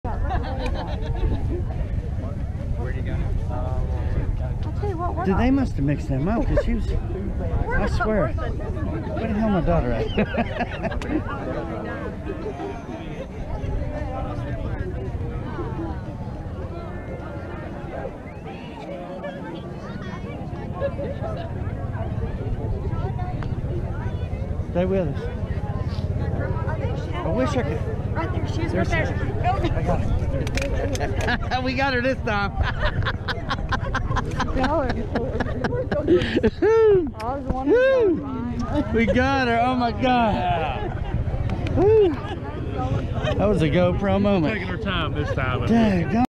Gopro Moment Tap Out/Teagan USAF
We had to tap Hanna out in order for her to leave with us after the graduation ceremony.